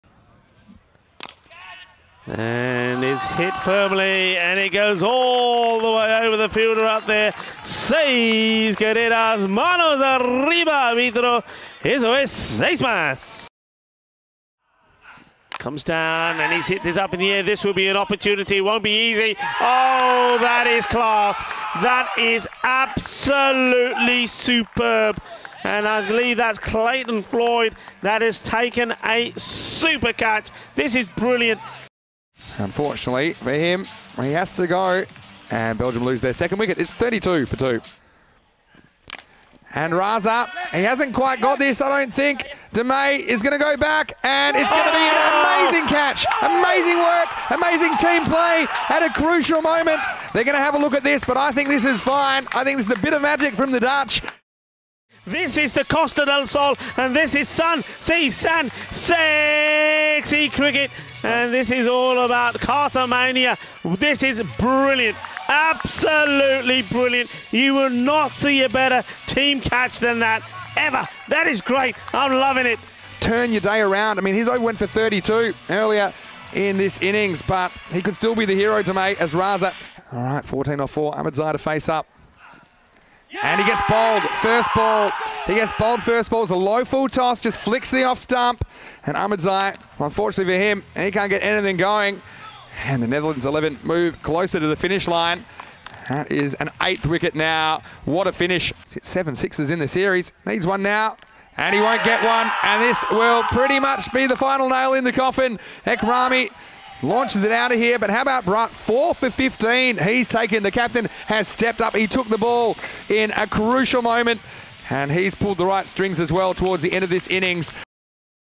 BRILLIANT Sebastian Braat BIG four wickets for Netherlands in derby win over Belgium
Man of the Match: Sebastian Braat (4/16) - BEL vs NED XI | Highlights | Dream11 European Cricket Championship Day 3 | ECC21.084